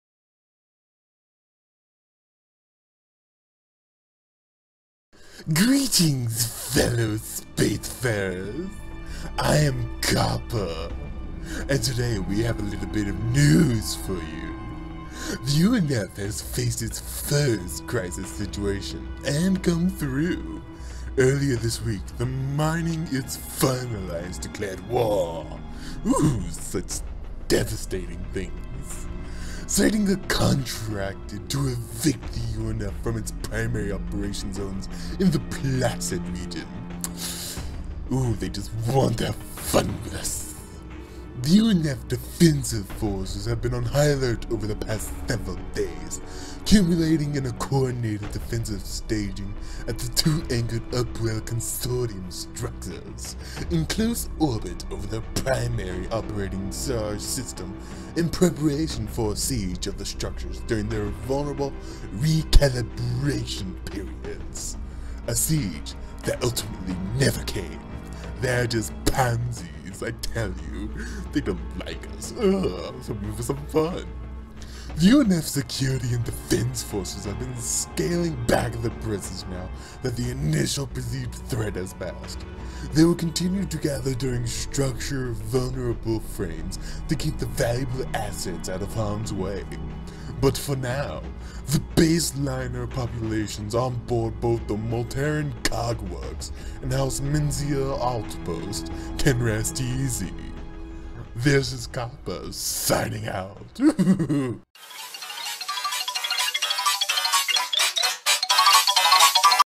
UNF report of the week.
The mining and security alliance gets a wardec.